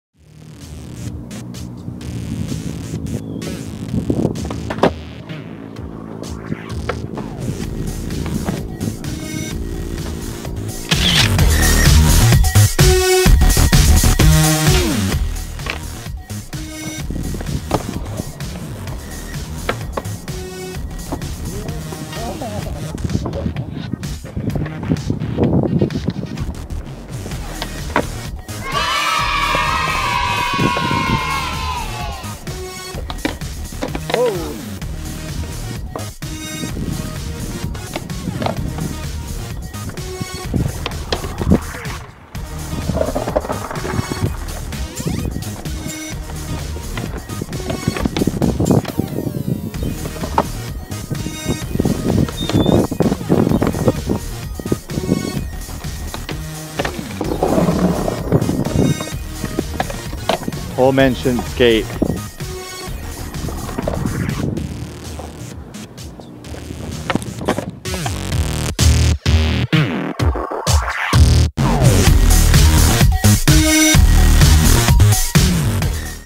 BuddyGripper Skateboarding - Volume 2
Pista de patinar en Madrid

Jay Hum and Chris McCoy are a little rusty but damn we give it a good effort!